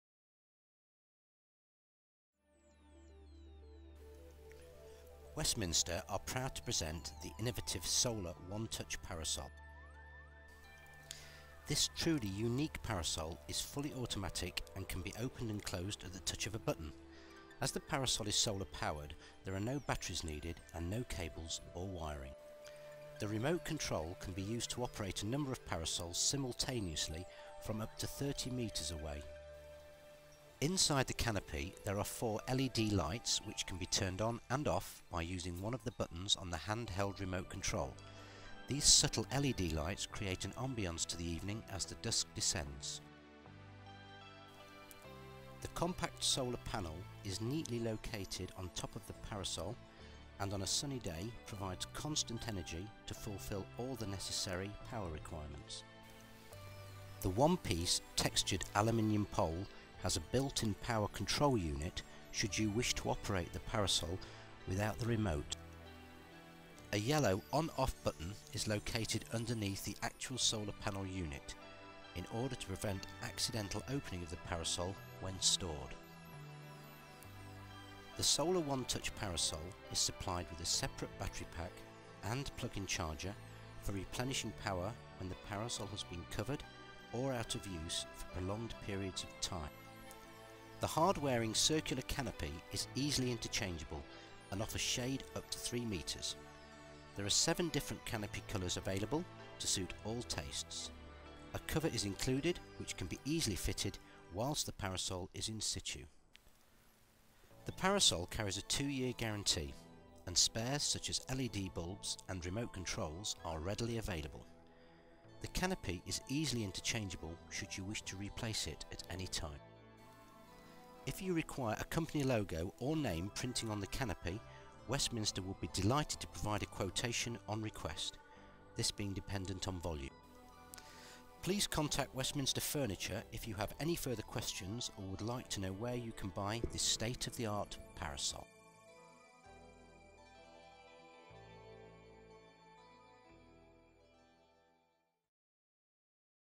Kingston Outdoors - Solar one touch
Kingston Outdoors presents the fantastic solar one touch parasol. A remote controlled parasol powered by solar panels give a unique and top quality touch to add to your outdoor furniture to enhance you outside living space.